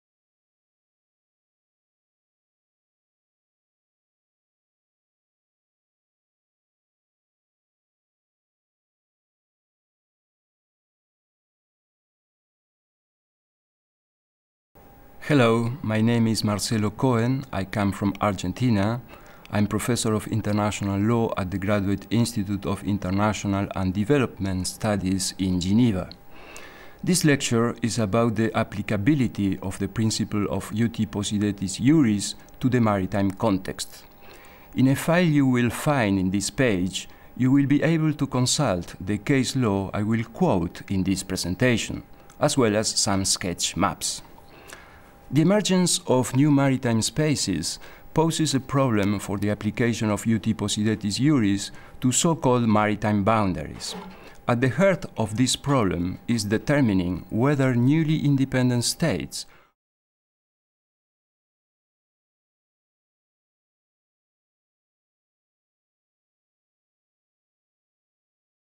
Marcelo Kohen on Uti Possidetis and Maritime Delimitations
Mr. Marcelo Kohen, Professor of International Law, Graduate Institute of International and Development Studies, Geneva